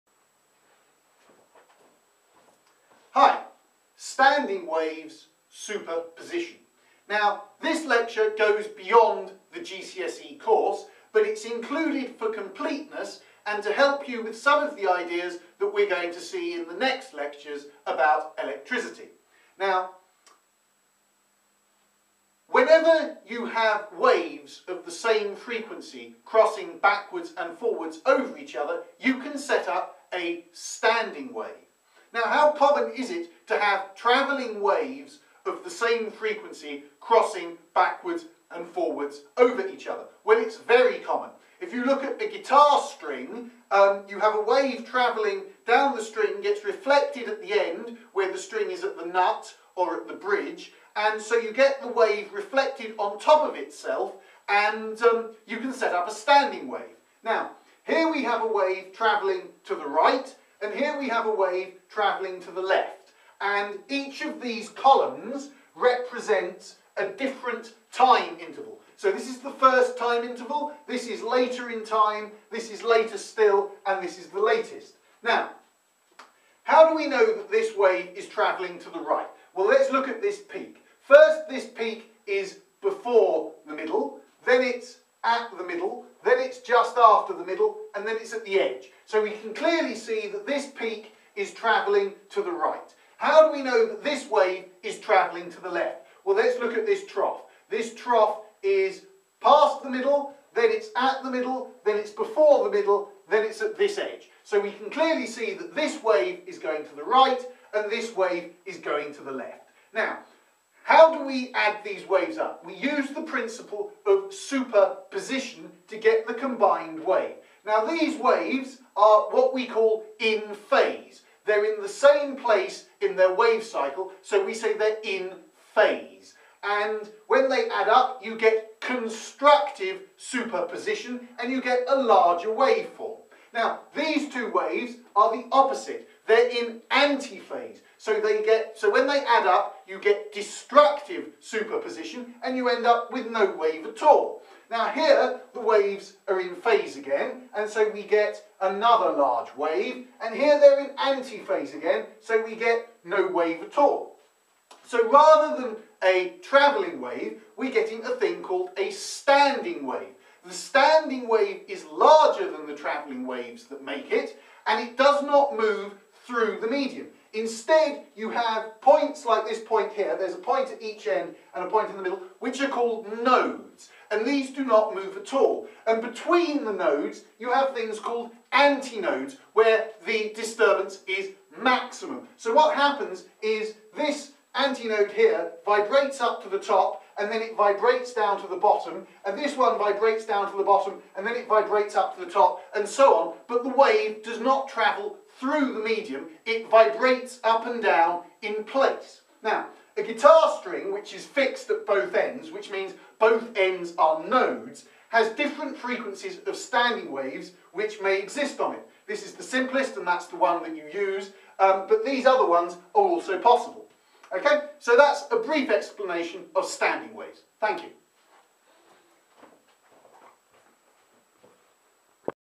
GCSE Physics Part 44 Standing Waves.MOV
A brief description of the phenomenon of Standing waves. Including ideas of phase and anti-phase. superposition, nodes and anti-nodes.  PLEASE NOTE this material is beyond GSCE Physics.